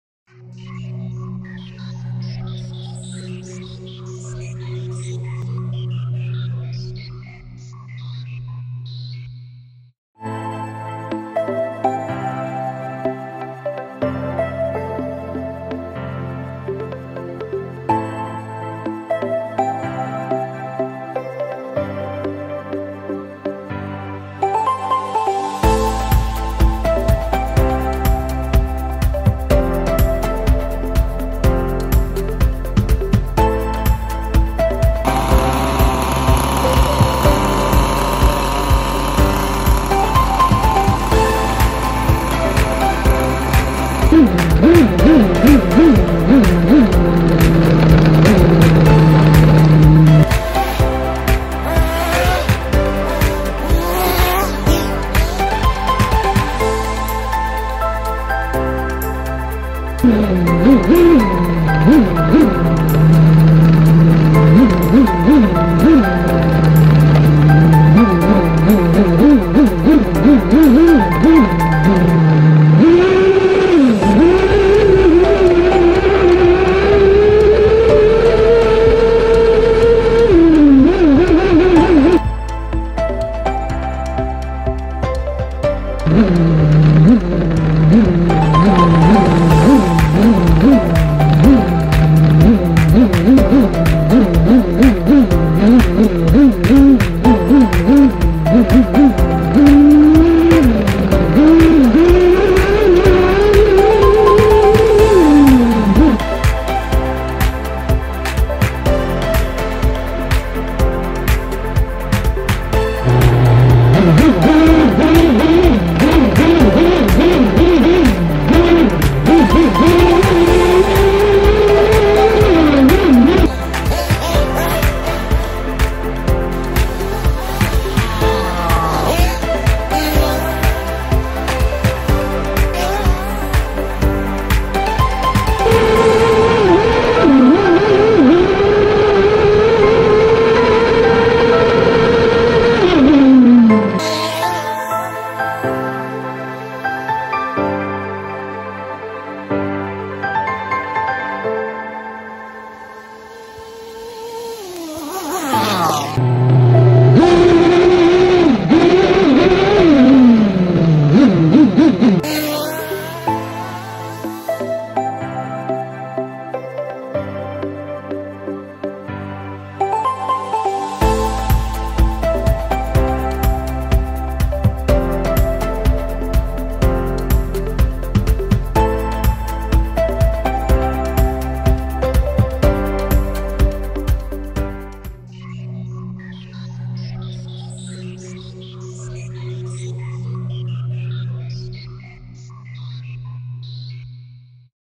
Kyosho MP7.5 US Sports Bashing
Took her out before I sell her. A little vid of it running at a local park. Cracking buggy.